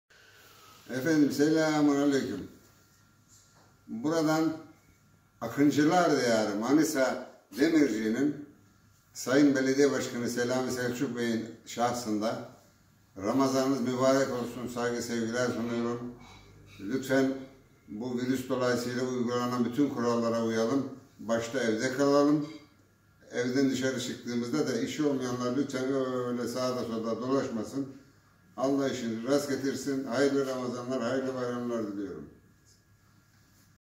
SANATÇI MUSTAFA KESER'DEN DEMİRCİ'YE SELAM
TRT Sanatçısı Mustafa Keser'den Manisa Demirci'ye Evde Kal çağrısı yaptı ve selam videosu gönderdi